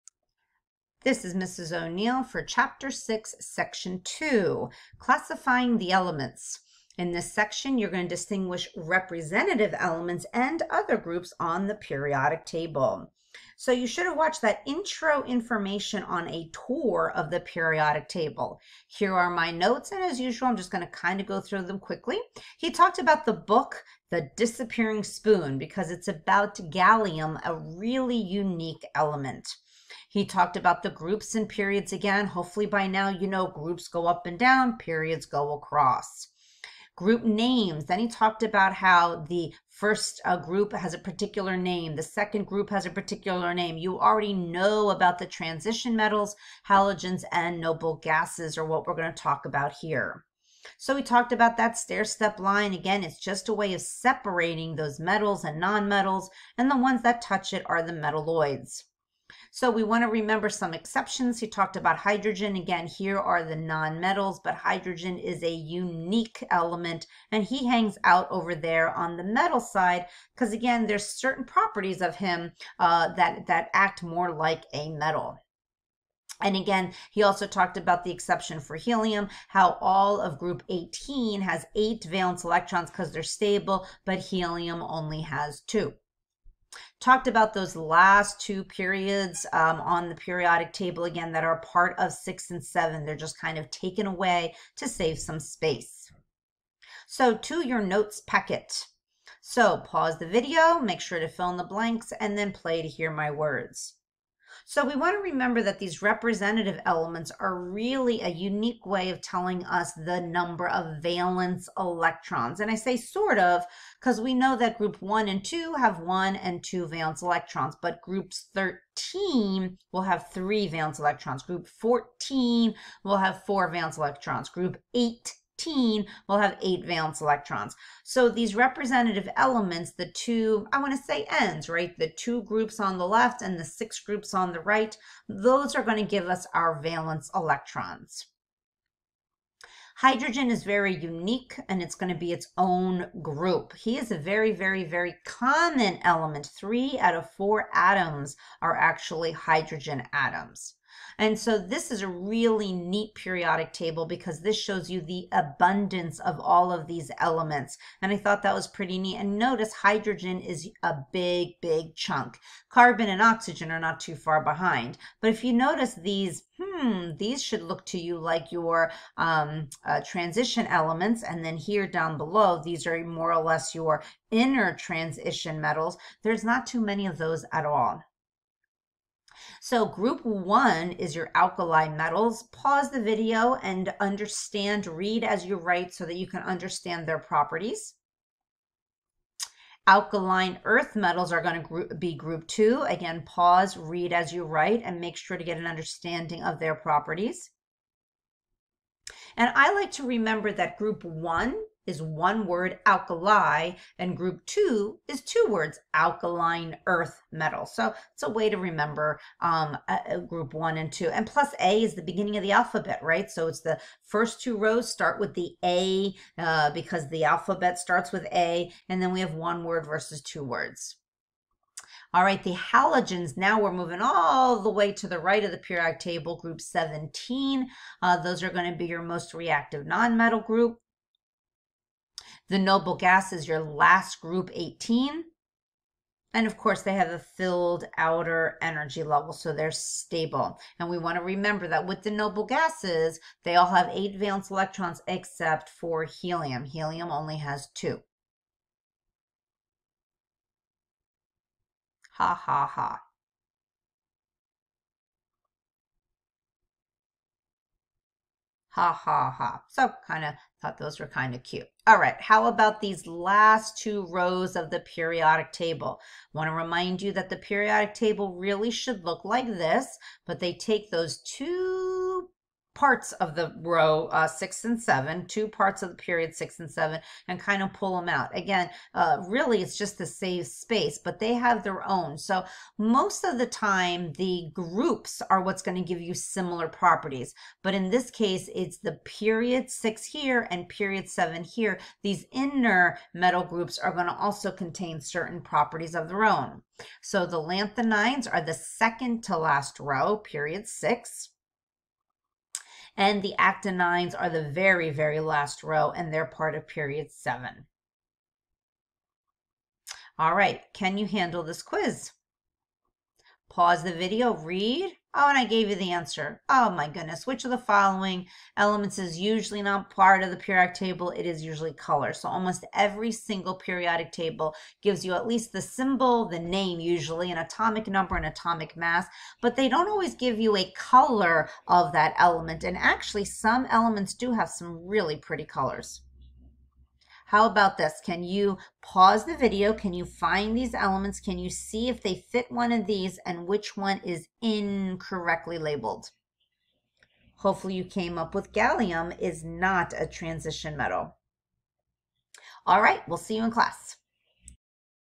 Organizing the Elements (Chemistry Section 6.2)
This video goes over group names on the Periodic Table including representative elements, hydrogen, Alkali metals, Alkaline Earth Metals, Halogen, Noble Gases, Lanthanides, and Actinides.